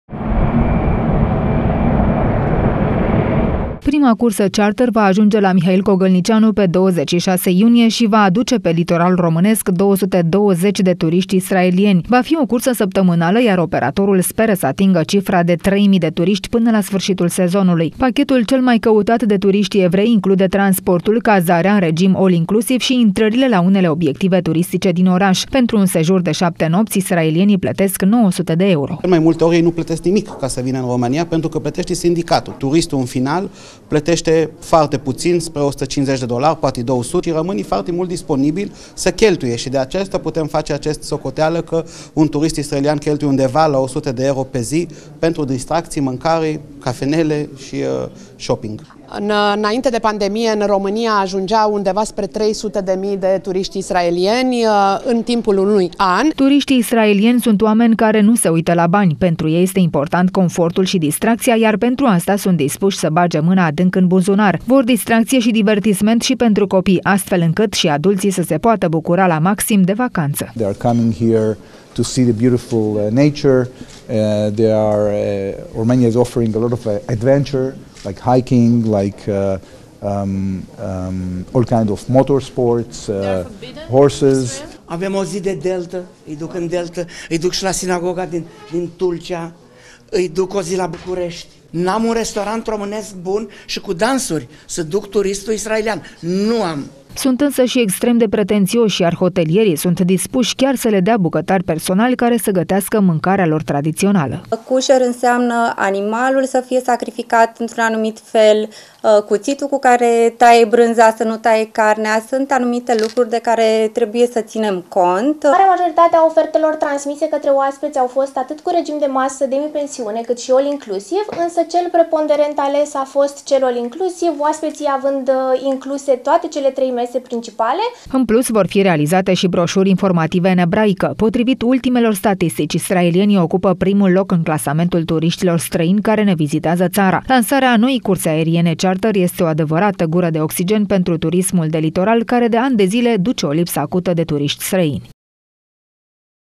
Turiștii israelieni se întorc pe litoralul românesc - LITORAL TV
Litoralul românesc își deschide din nou porțile pentru turiștii israelieni. După o pauză de trei ani cauzată și de pandemia de Covid, sute de turiști vor ajunge în hotelurile de 4 și 5 stele din stațiunile românești de la Marea Neagră. În trei ore aceștia vor pune piciorul pe pământ românesc datorită cursei charter Tel Aviv – Constanța – Tel Aviv. Pentru un sejur de 7 nopți pe litoral un turist israelian va plăti în jur de 900 de euro.